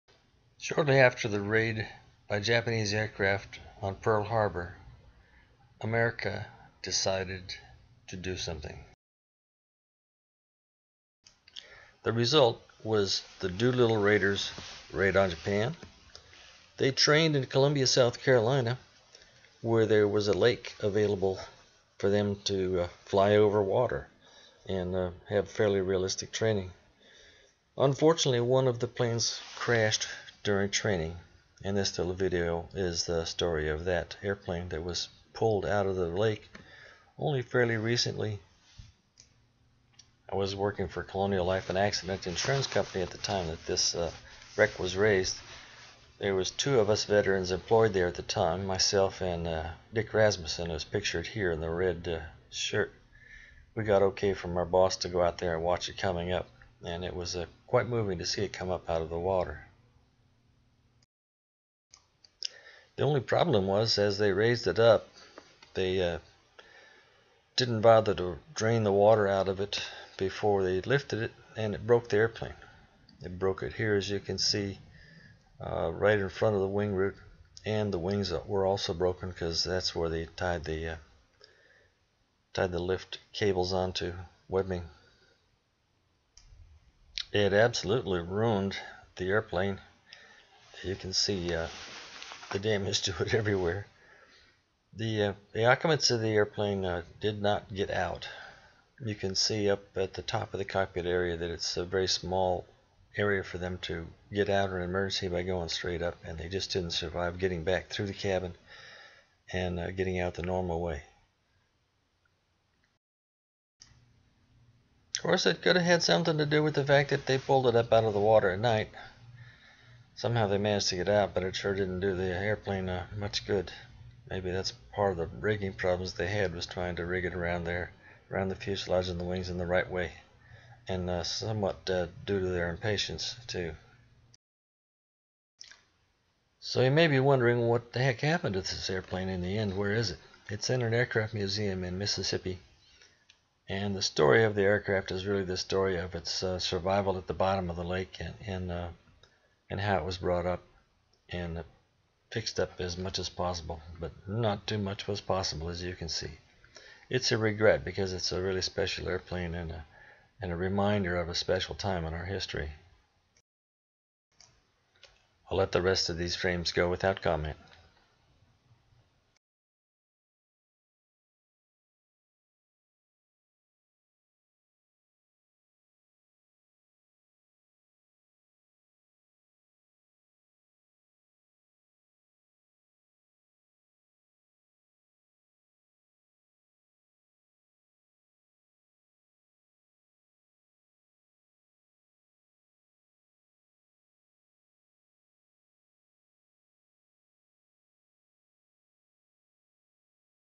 Dolittle Raider aircraft salvaged
This aircraft never flew it the raid.  During a training flight, it crashed into a lake near Columbia, South Carolina.